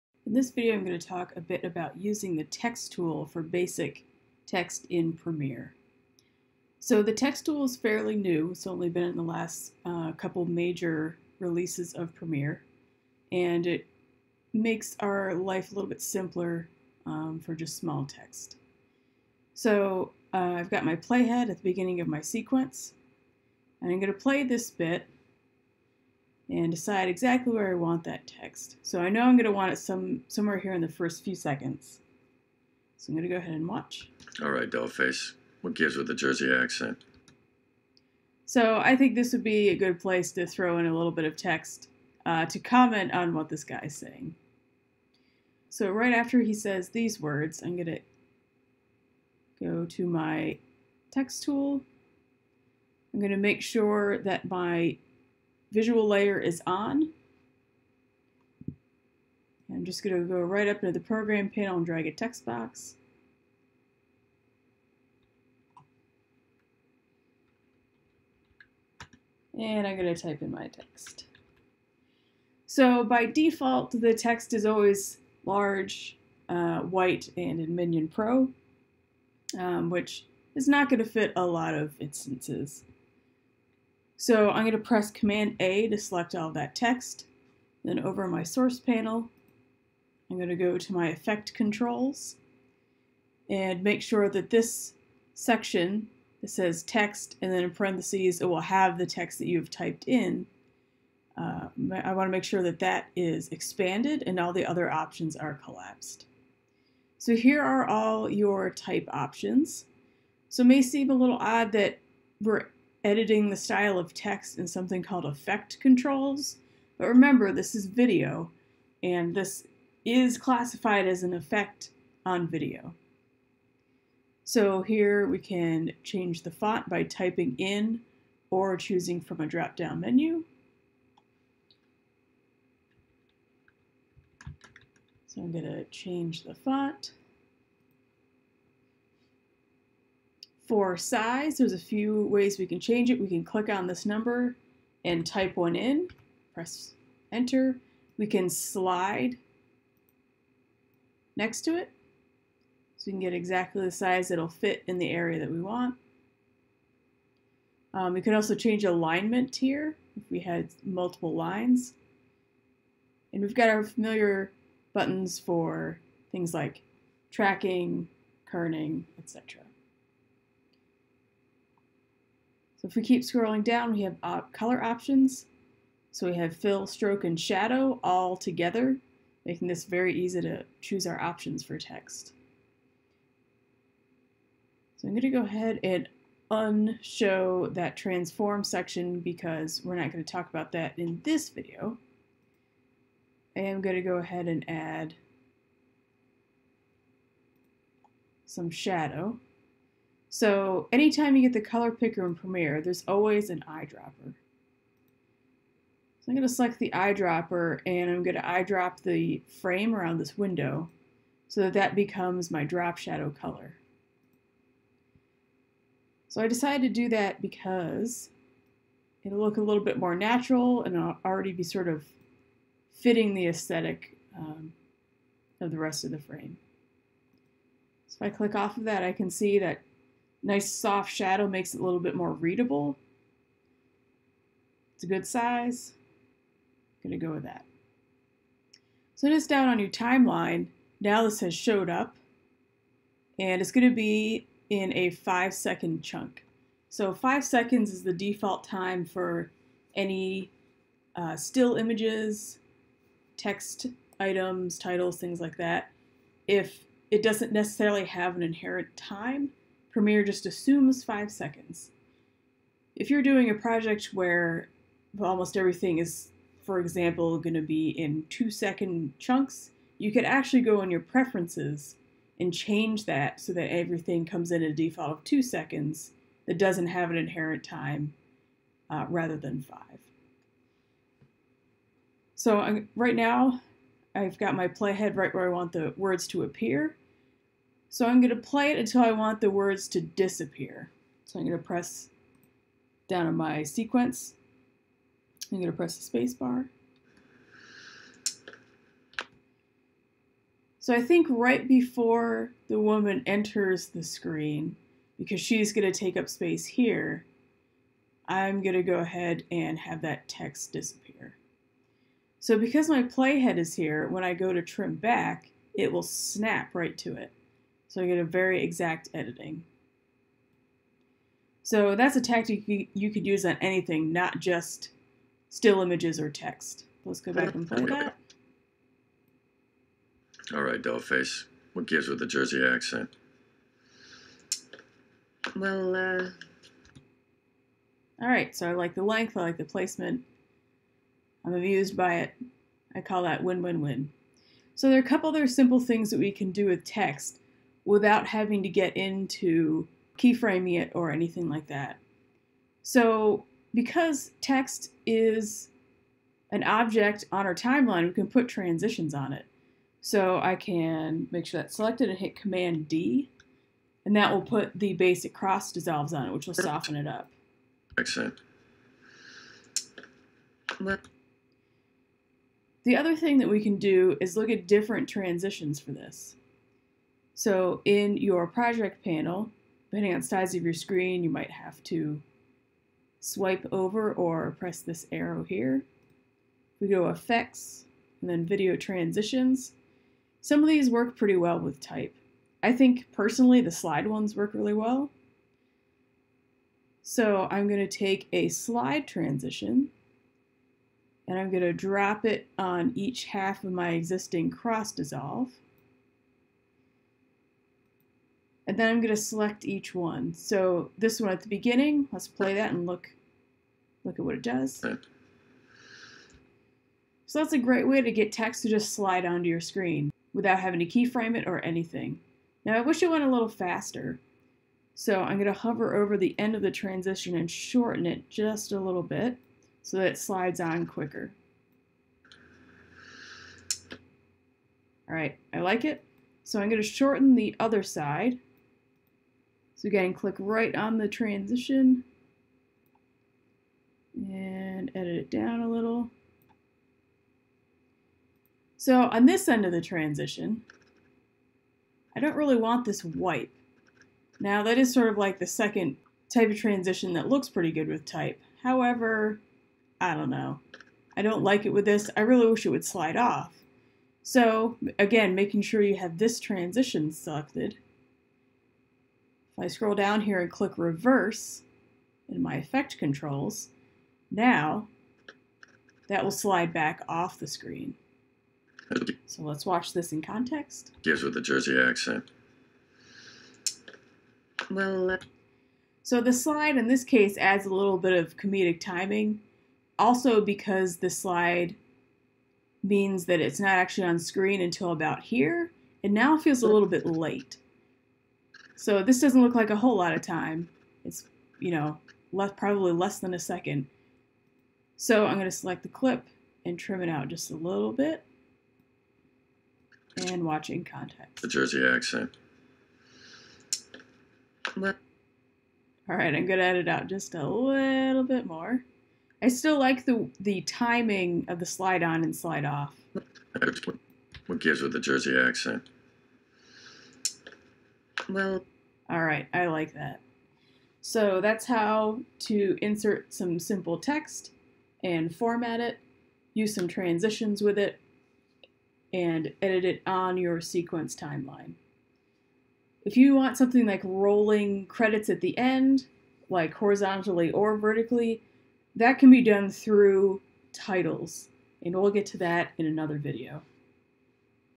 Premiere Pro - Using the Type Tool
Create and edit basic type/text elements in Premiere Pro.